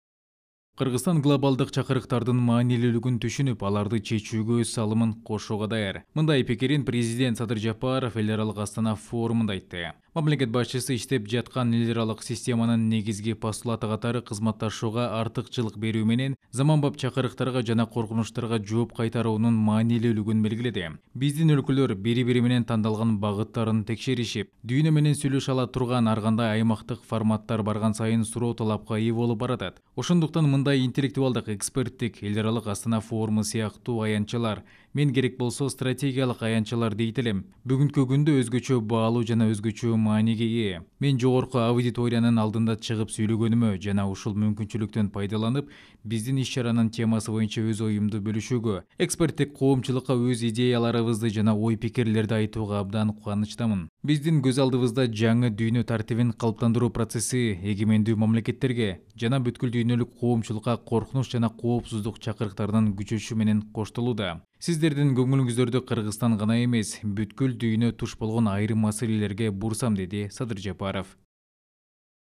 Кыргызстан глобалдык чакырыктарды чечүүгө салым кошууга даяр
Кыргызстан глобалдык чакырыктардын маанилүүлүгүн түшүнүп, аларды чечүүгө өз салымын кошууга даяр. Мындай пикирин президент Садыр Жапаров эл аралык Астана форумунда айтты. Мамлекет башчысы иштеп жаткан эл аралык системанын негизги постулаты катары кызматташууга артыкчылык берүү менен заманбап чакырыктарга жана коркунучтарга жооп кайтаруунун маанилүүлүгүн белгиледи.
"Биздин өлкөлөр бири-бири менен тандалган багыттарын текшеришип, дүйнө менен сүйлөшө ала турган ар кандай аймактык форматтар барган сайын суроо-талапка ээ болуп баратат. Ошондуктан, мындай интеллектуалдык, эксперттик Эл аралык Астана форуму сыяктуу аянтчалар, мен керек болсо стратегиялык аянтчалар дейт элем, бүгүнкү күндө өзгөчө баалуу жана өзгөчө мааниге ээ. Мен жогорку аудиториянын алдында чыгып сүйлөгөнүмө жана ушул мүмкүнчүлүктөн пайдаланып, биздин иш-чаранын темасы боюнча өз оюмду бөлүшүүгө, эксперттик коомчулукка өз идеяларыбызды жана ой-пикирлерди айтууга абдан кубанычтамын. Биздин көз алдыбызда жаңы дүйнө тартибин калыптандыруу процесси эгемендүү мамлекеттерге жана бүткүл дүйнөлүк коомчулукка коркунуч жана коопсуздук чакырыктарынын күчөшү менен коштолууда. Сиздердин көңүлүңүздөрдү Кыргызстан гана эмес, бүткүл дүйнө туш болгон айрым маселелерге бурсам", - деди Садыр Жапаров.